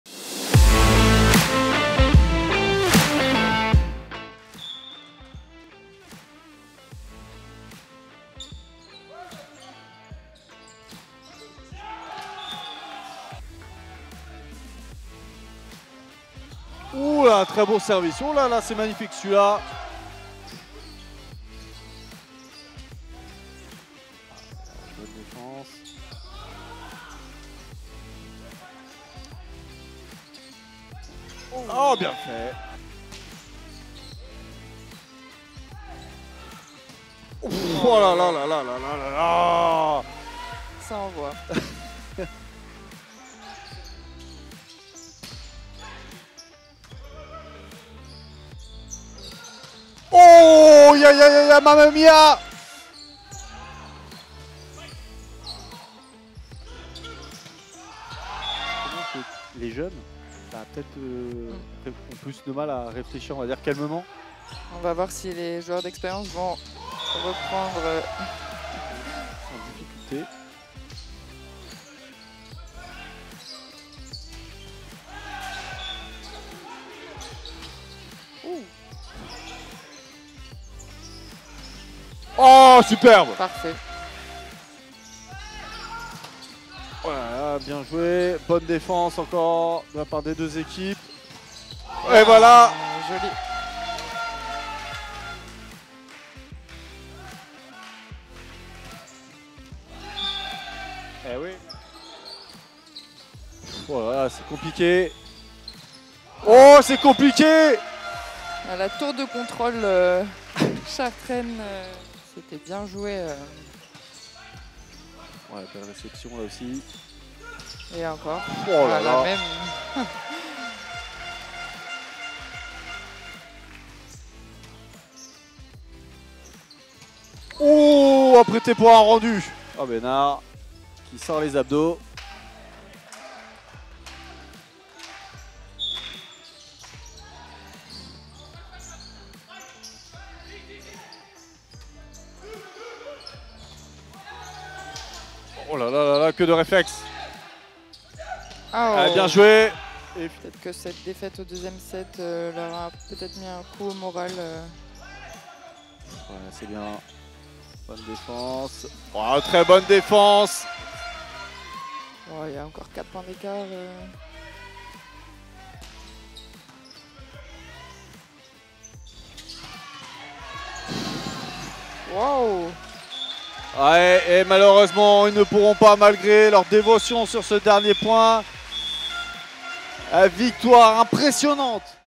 [Temps forts] C'Chartres Volley vs Saint-Nazaire
Impressionnant ! Dans ce choc du haut de tableau, le C'Chartres Volley n'a laissé aucun espoir à ST NAZAIRE VB ATLANTIQUE 2 avec le score de 3-0. Le CCV conforte sa deuxième place à 1 point du leader.

Rendez-vous le 27 novembre pour le prochain match à domicile du C'Chartres Volley face au Volley Ball Pexinois Niort